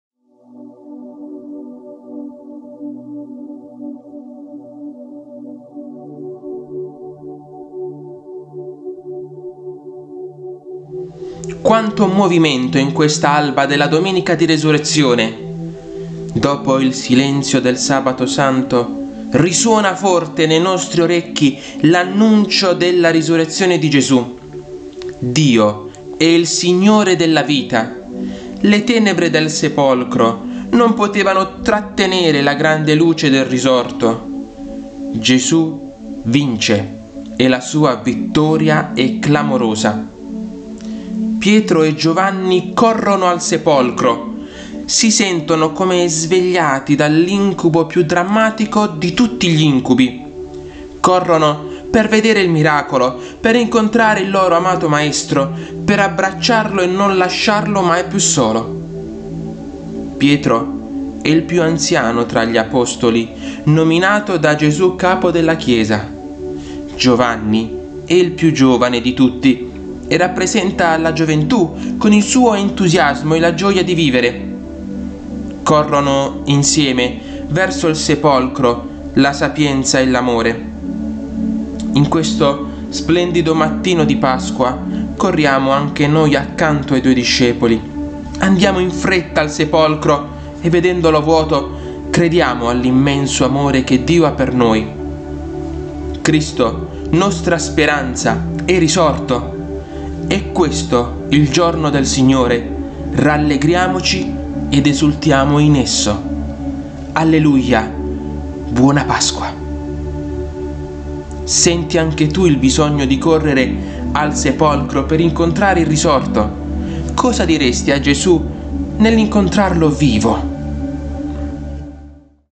⏩ PAROLE DI QUARESIMA 2023 | 9 aprile
Domenica di Pasqua – 9 aprile

Quanto movimento in questa alba della Domenica di Resurrezione! Dopo il silenzio del Sabato Santo risuona forte nei nostri orecchi l'annuncio della risurrezione di Gesù.
Dio è il Signore della vita. Le tenebre del sepolcro non potevano trattenere la grande luce del Risorto. Gesù vince e la sua vittoria è clamorosa.
Pietro e Giovanni corrono al sepolcro. Si sentono come svegliati dall'incubo più drammatico di tutti gli incubi. Corrono per vedere il miracolo, per incontrare il loro amato Maestro, per abbracciarlo e non lasciarlo mai più solo.
Pietro è il più anziano tra gli apostoli, nominato da Gesù capo della Chiesa. Giovanni è il più giovane di tutti e rappresenta la gioventù con il suo entusiasmo e la gioia di vivere. Corrono insieme, verso il sepolcro, la sapienza e l'amore.
In questo splendido mattino di Pasqua corriamo anche noi accanto ai due discepoli. Andiamo in fretta al sepolcro e, vedendolo vuoto, crediamo all'immenso amore che Dio ha per noi.
Cristo nostra speranza è risorto! È questo il giorno del Signore, rallegriamoci ed esultiamo in esso. Alleluia! Buona Pasqua!

Per riflettere ...
▪ Senti anche tu il bisogno di correre al sepolcro per incontrare il Risorto?
▪ Cosa diresti a Gesù nell’incontrarlo vivo?

Riflessione tratta da libro «Quaresima 2023 giorno per giorno: parola, riflessioni, proposito»